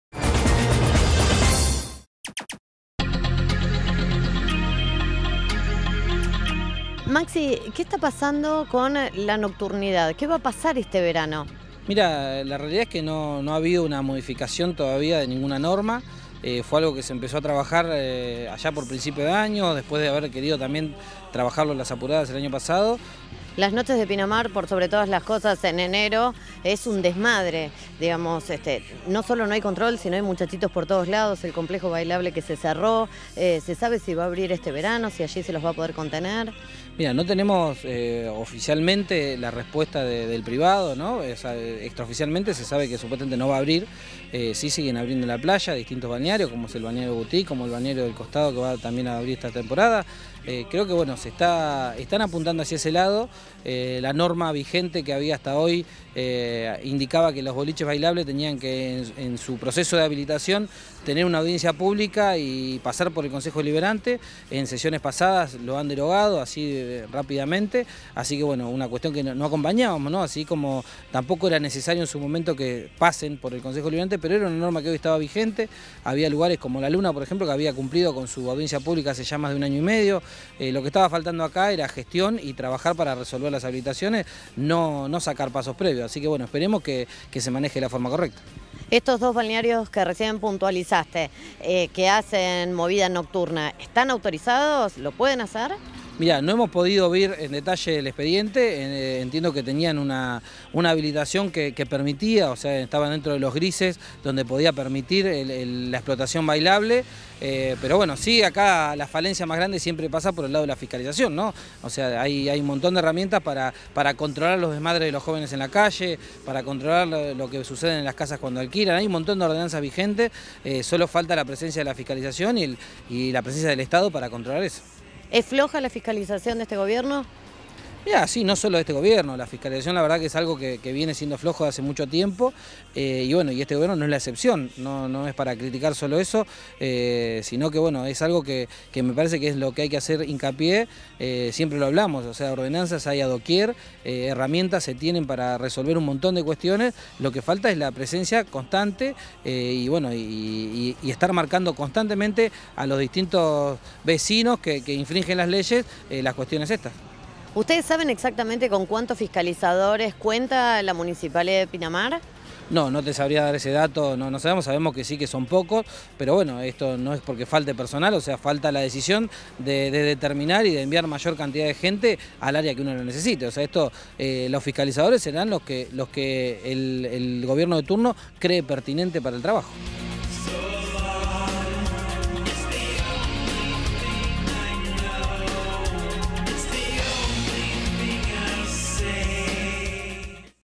LA NOCTURNIDAD EN EL VERANO
Ante el cierre de los complejos bailables en la pasada temporada, poco se ha hecho durante 2017 en el HCD para controlar la nocturnidad  para que los jóvenes estén contenidos y los comerciantes tengan parámetros regidos por ordenanza.El concejal Maximiliano Santini da detalles del relativo vacío legal por segundo verano consecutivo con un tema que preocupa a todos